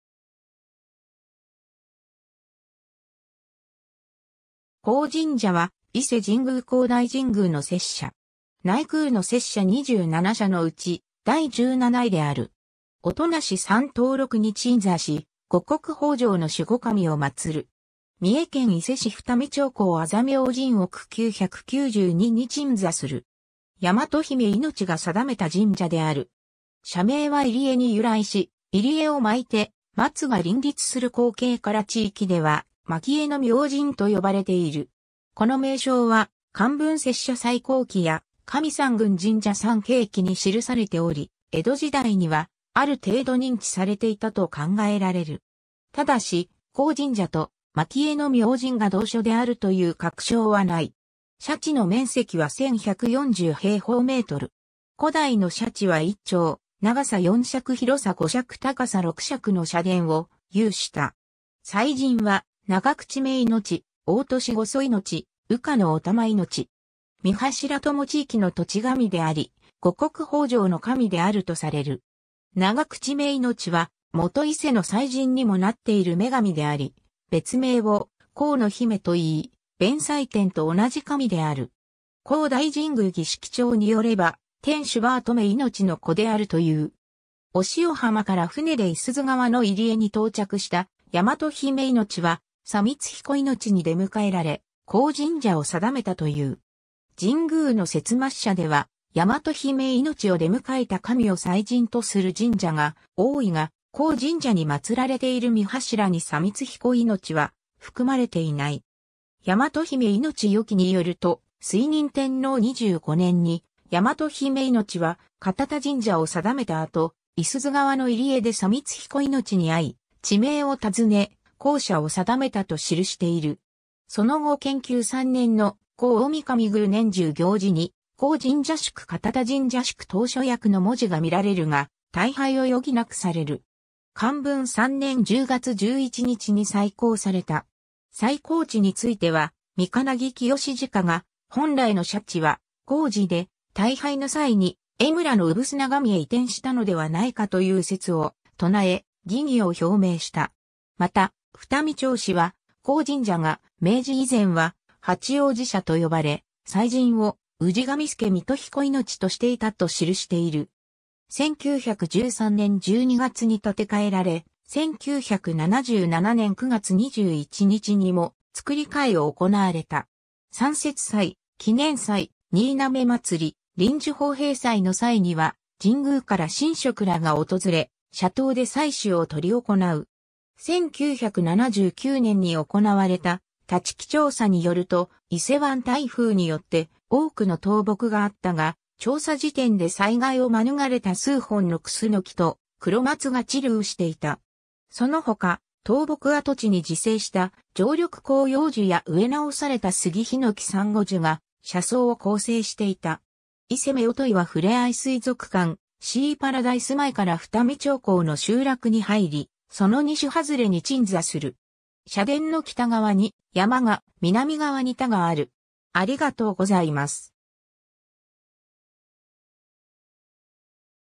江神社
江神社（えじんじゃ）は、伊勢神宮皇大神宮（内宮）の摂社。
内宮の摂社27社のうち第17位である。
 音無山東麓に鎮座し、五穀豊穣の守護神を祀る。
 三重県伊勢市二見町江字明神奥992に鎮座する。
倭姫命が定めた神社である。
 社名は入り江に由来し、入り江を巻いて松が林立する光景から地域では「巻江の明神」（蒔絵の明神）と呼ばれている。
この名称は『寛文摂社再興記』や『神三郡神社参詣記』に記されており、江戸時代にはある程度認知されていたと考えられる。
ただし、江神社と「巻江の明神」が同所であるという確証はない。
 社地の面積は1,140m2。
古代の社地は1町（≒9,917m2）、長さ4尺（≒1.2m）×広さ5尺（≒1.5m）×高さ6尺（≒1.8m）の社殿を有した。
 祭神は、長口女命（ながくちめのみこと）、大歳御祖命（おおとしのみおやのみこと）、宇加乃御玉命（うかのみたまのみこと）。
3柱とも地域の土地神であり、五穀豊穣の神であるとされる。
 長口女命は元伊勢の祭神にもなっている女神であり、別名を「江之姫」と言い、弁才天と同じ神である。
『皇大神宮儀式帳』によれば、天須婆留女命（あめのすばるのみこと、棒原神社の祭神）の子であるという。
 御塩浜から船で五十鈴川の入り江に到着した倭姫命は、佐見津日子命に出迎えられ、江神社を定めたという。
神宮の摂末社では、倭姫命を出迎えた神を祭神とする神社が多いが、江神社に祀られている3柱に佐見津日子命は含まれていない。
『倭姫命世記』によると、垂仁天皇25年に倭姫命は堅田神社を定めた後、五十鈴川の入り江で佐見津日子命に会い、地名を尋ね、「江社」を定めたと記している。
その後建久3年（1192年）の『皇太神宮年中行事』に「江神社祝堅田神社祝等所役」の文字が見られるが、退廃を余儀なくされる。
 寛文3年10月11日（グレゴリオ暦：1663年11月10日）に再興された。
再興地については御巫清直が本来の社地は江寺（現・太江寺）で、退廃の際に江村の産土神（現・栄野神社）へ移転したのではないかという説を唱え、疑義を表明した。
また『二見町史』は、江神社が明治以前は「八王子社」と呼ばれ、祭神を「氏神佐美都彦命」としていたと記している。
 1913年（大正2年）12月に建て替えられ、1977年（昭和52年）9月21日にも造り替えを行われた。
 三節祭（月次祭・神嘗祭）、祈年祭、新嘗祭、臨時奉幣祭の際には神宮から神職らが訪れ、社頭で祭祀を執り行う。
 1979年（昭和54年）に行われた立木調査によると、伊勢湾台風によって多くの倒木があったが、調査時点で災害を免れた数本のクスノキとクロマツが散生していた。
そのほか、倒木跡地に自生した常緑広葉樹や植え直されたスギ・ヒノキ・サンゴジュが社叢を構成していた。
 伊勢夫婦岩ふれあい水族館シーパラダイス前から二見町江の集落に入り、その西外れに鎮座する。
社殿の北側に山が、南側に田がある。